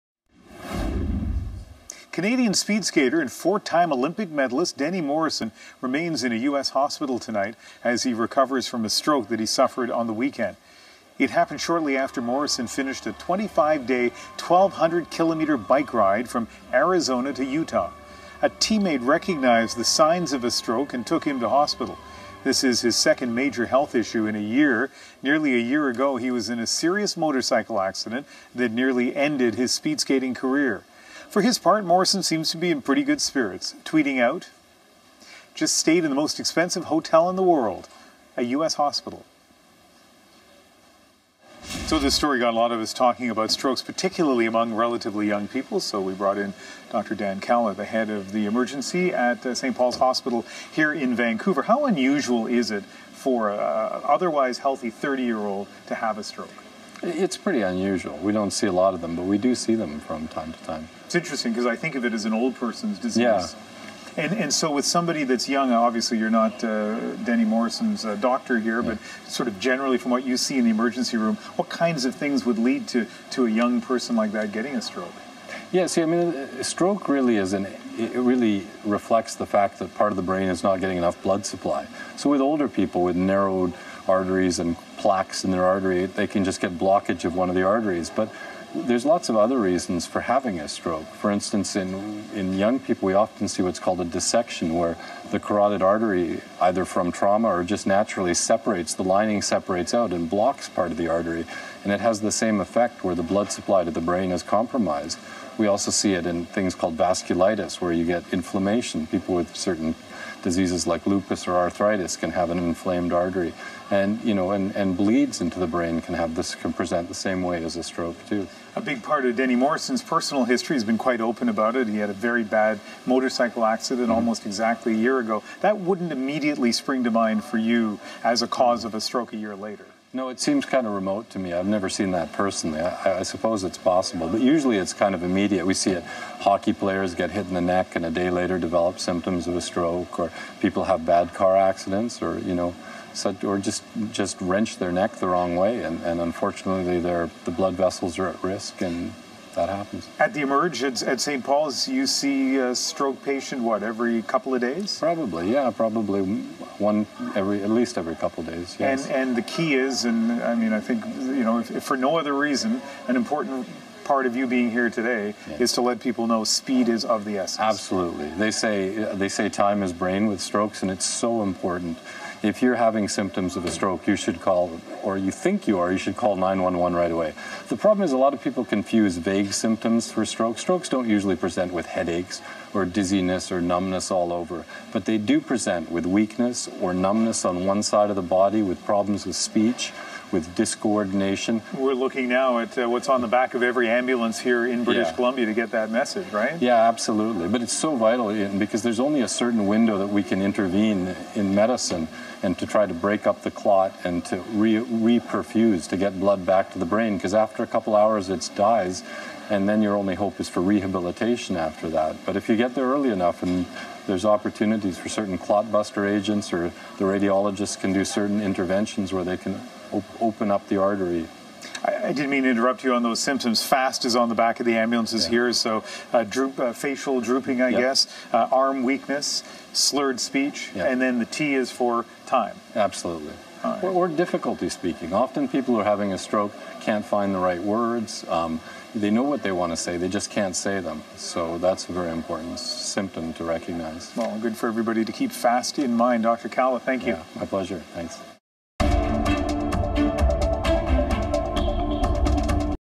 Cdn Speedskater Denny Morrison recovering from a stroke.
We talk to Dr Dan  Kalla, the head of St. Paul's Emergency in Vancouver about the warning signs of strokes and how strokes occur in younger  people more often than you might think.